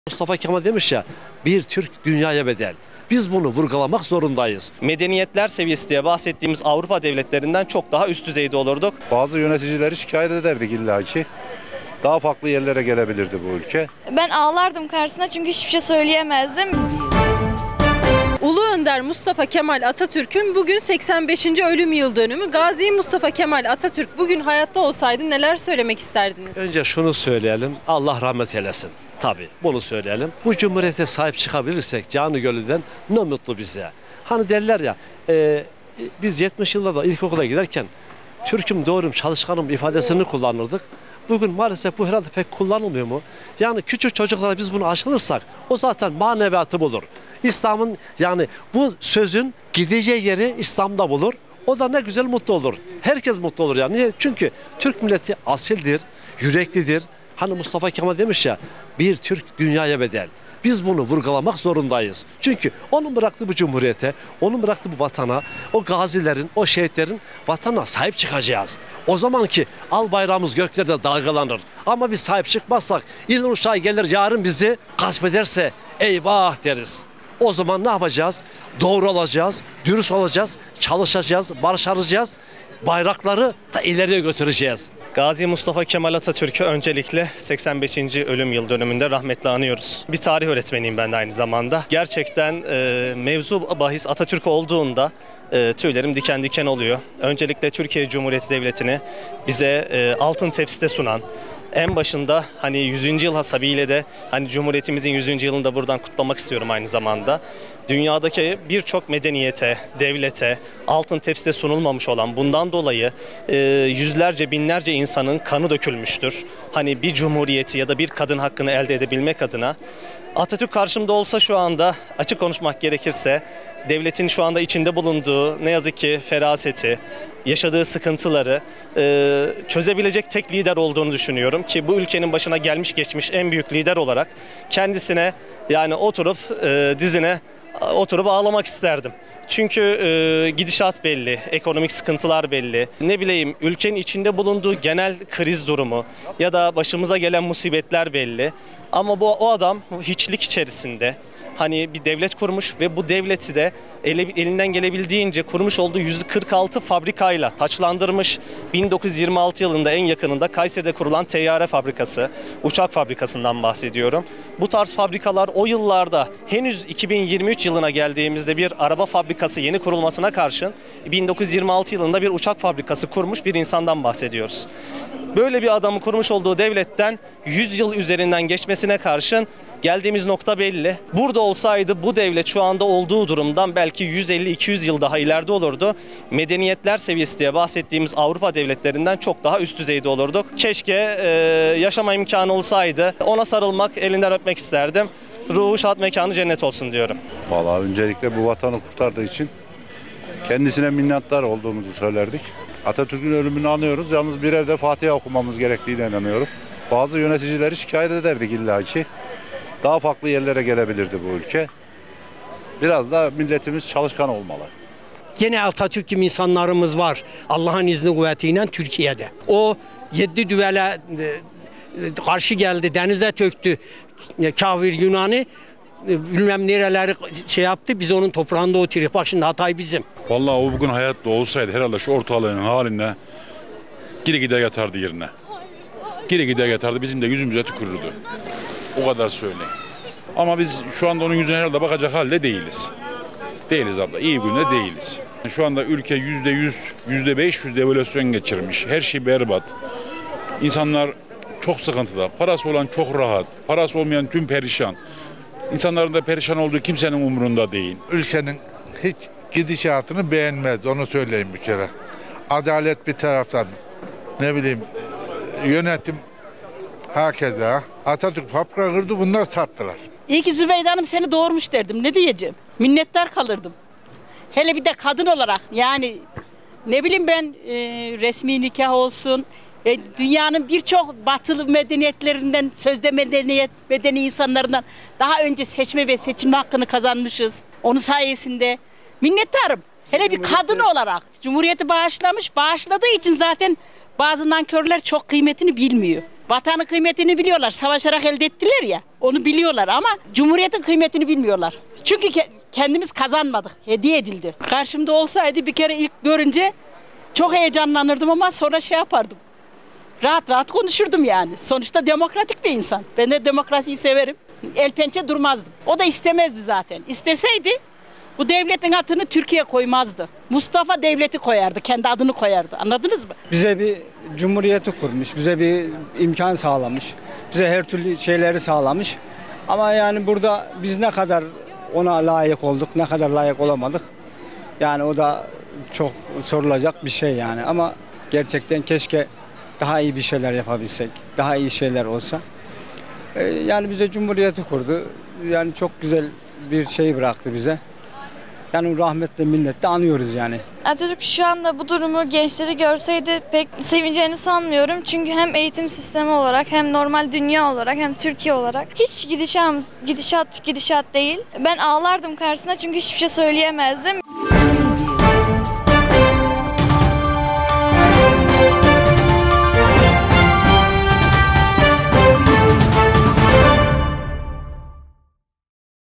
Atatürk bugün hayatta olsa neler söylemek isterdiniz?
Ulu önder Mustafa Kemal Atatürk'ün bugün 85. ölüm yıl dönümü. Gazi Mustafa Kemal Atatürk bugün hayatta olsa neler söylemek isterdiniz?